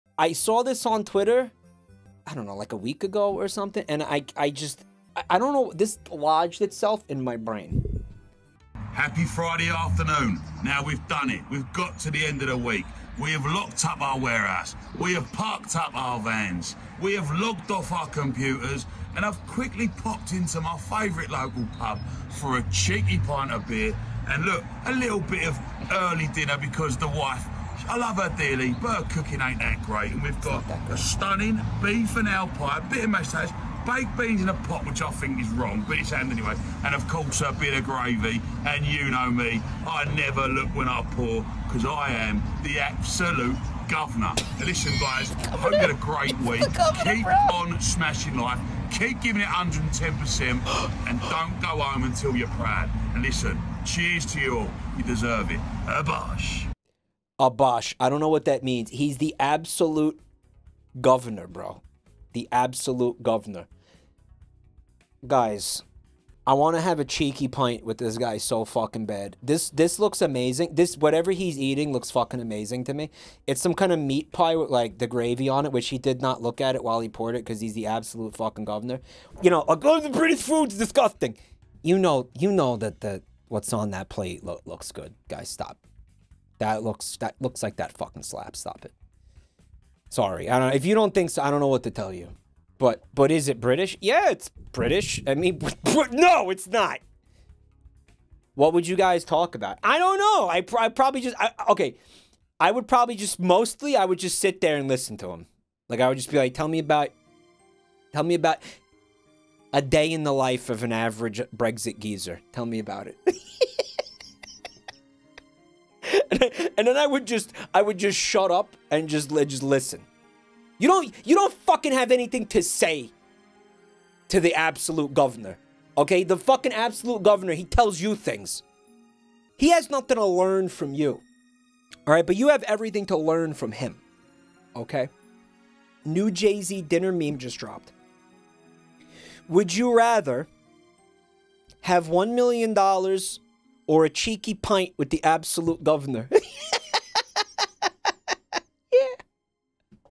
Absolute Governor
Mike would like to get a pint with the absolute governor

Highlights: @Jabroni_Mike
Full Streams: @JabroniMikeFullStreams
Clips: @jabronimikeclips